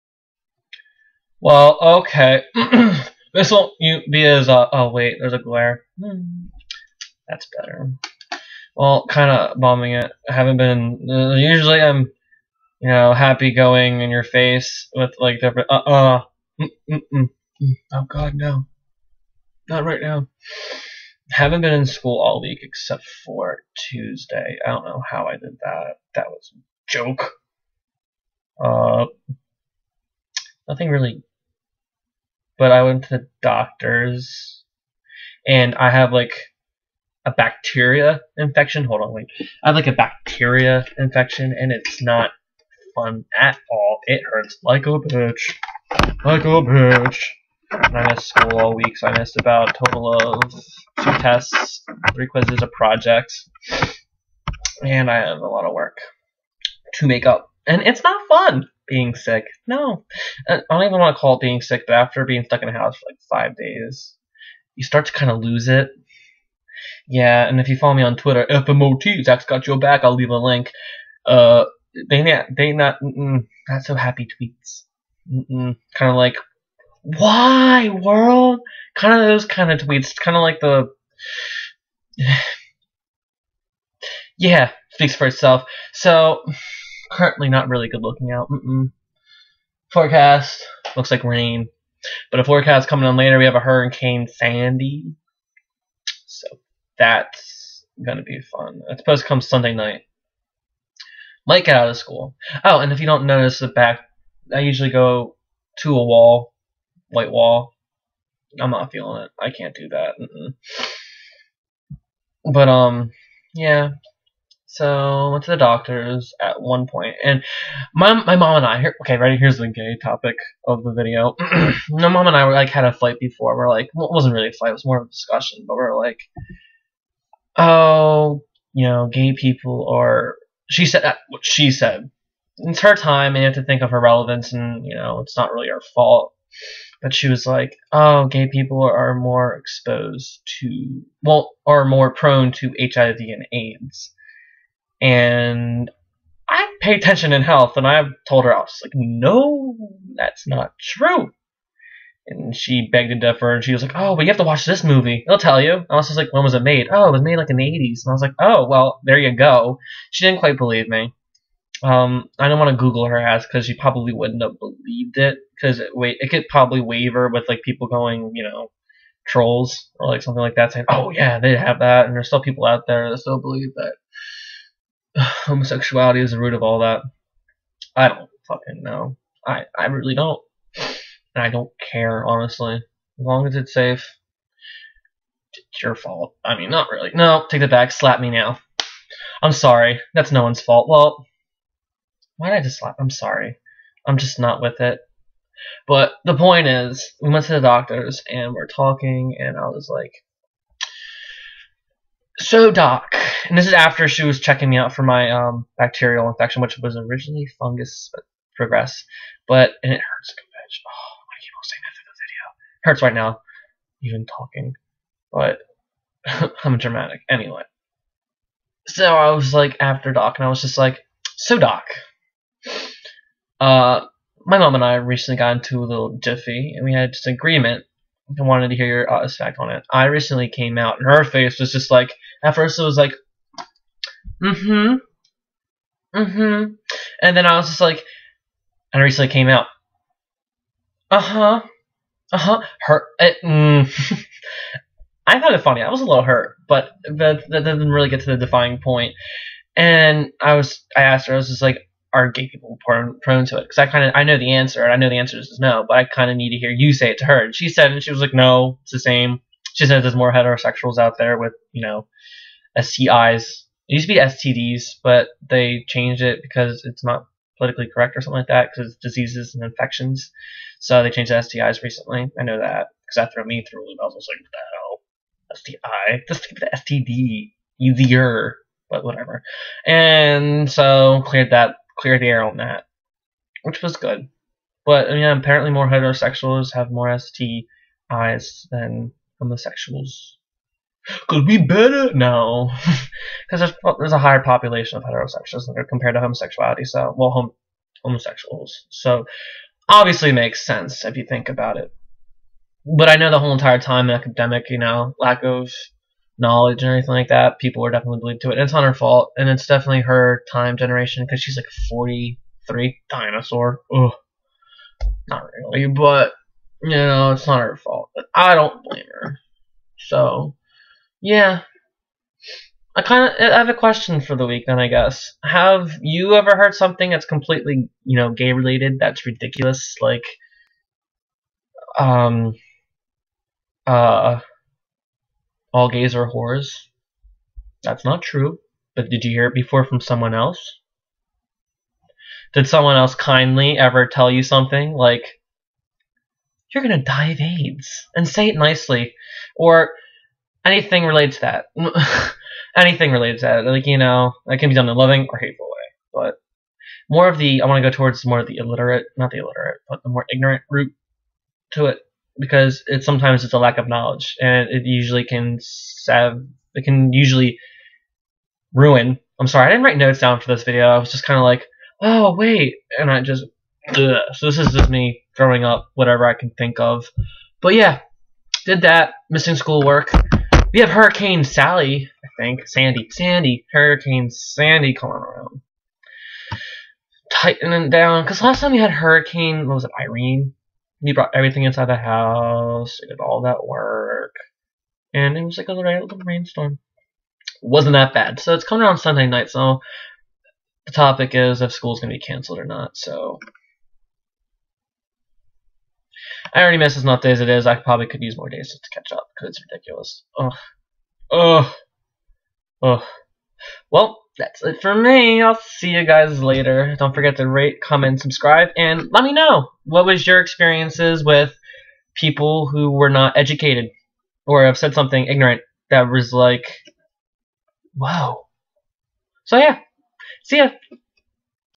Gay Teen High School Vlog 6
heey guys...sorry...this just isn't my week. Shield your eyes from my complete sickly state. Literally losing it, haha and omg it's a scab next to my mouth, just try to ignore it. (I know I cant)